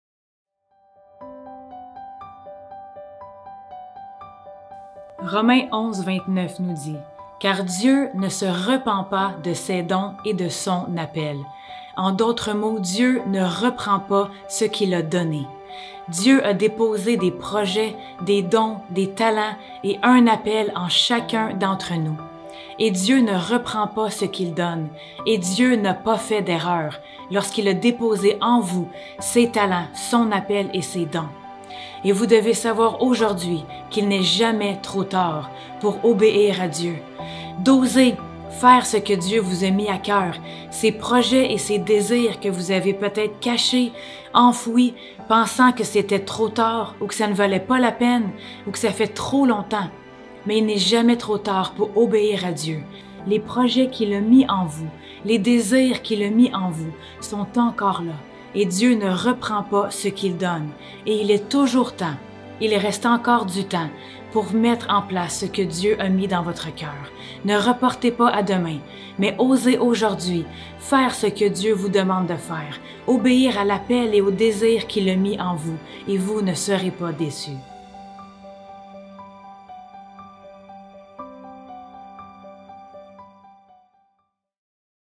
Semence quotidienne Romains 11:29
Romains 11:29 Car Dieu ne se repent pas de ses dons et de son appel.